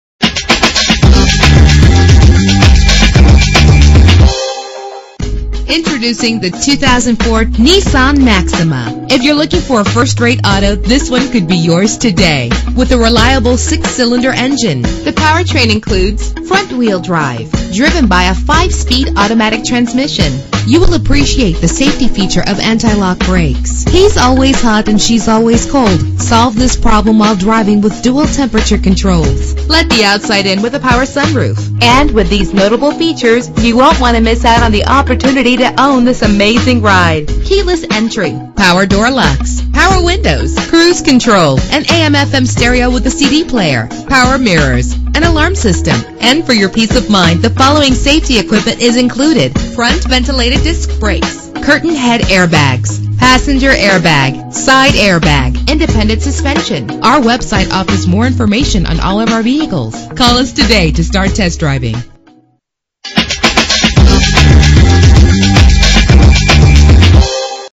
2004 Nissan Maxima - Old Bridge NJ
Year: 2004
Make: Nissan
Model: Maxima
Trim: 3.5 SE
Engine: 3.5 liter 6 cylinder 24 valve
Transmission: 5-Speed Automatic
Color: Winter Frost Pearl
Mileage: 101912
Address:
1101 US highway 9